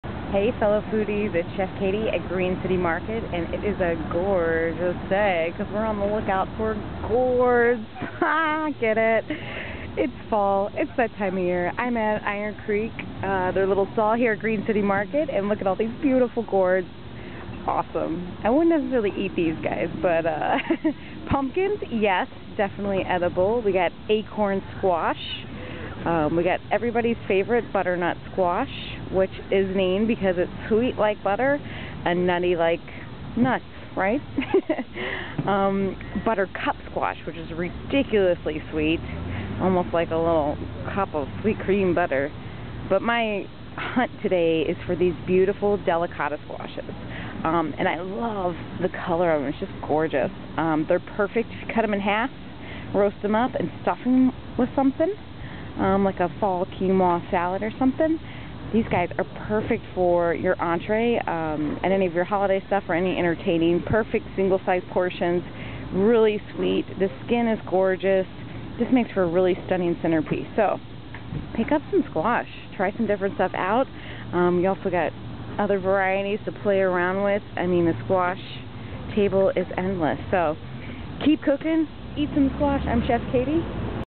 Fall Squash 1
Join Chef Katie at the market as she explores the gourds!  There are so many fall squash varieties to try!  Pick one up and start cooking.